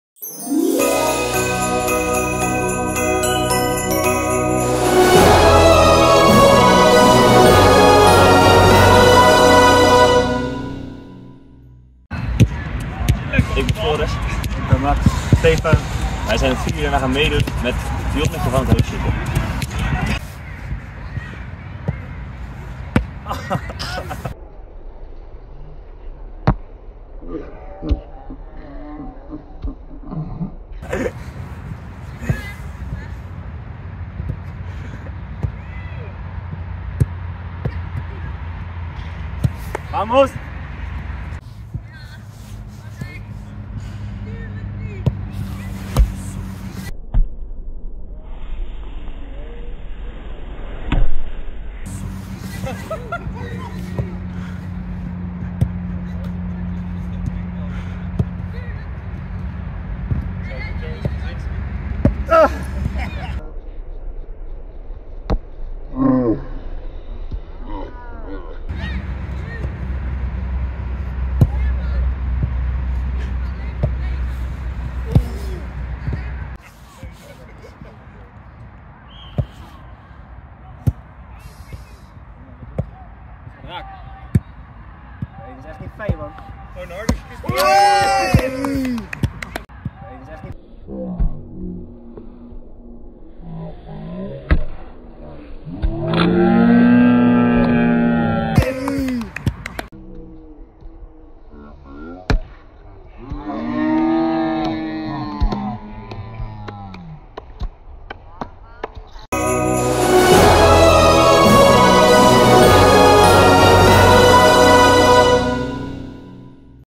Pion van het hoofd schieten | 1 december 2022 | Adventskalender Excelsior Maassluis
Iedere dag een nieuwe video, tot aan de Kerst. Met challenges, spelletjes en nog veel meer!  Vandaag is het de beurt aan Excelsior Maassluis 4 met 'Pion van het hoofd schieten'. Gaat het ze lukken?